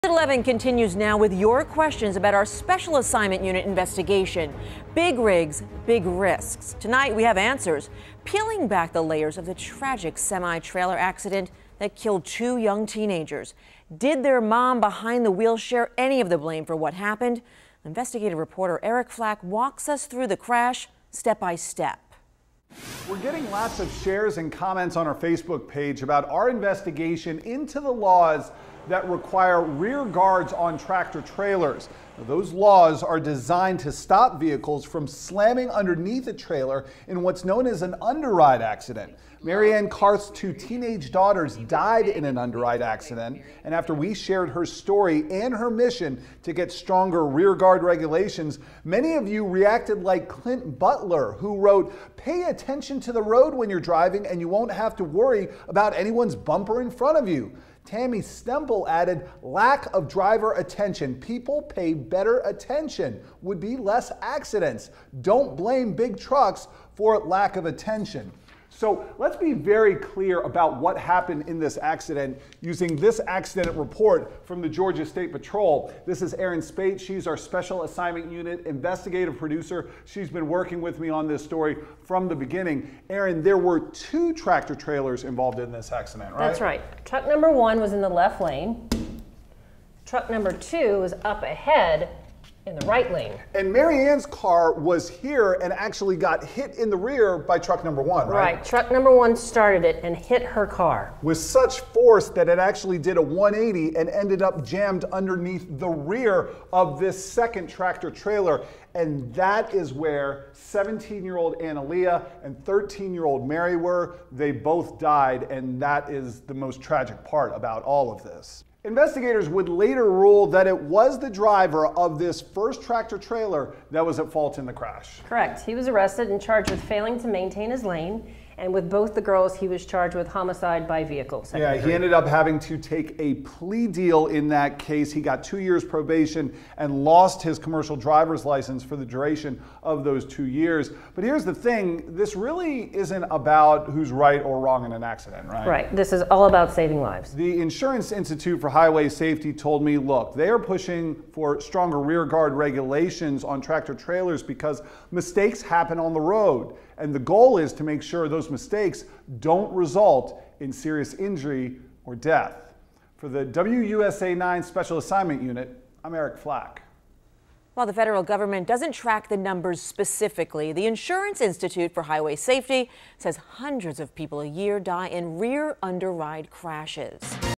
Rear underride accidents explained
The insurance institute for highway safety says hundreds of people a year die in rear underride crashes.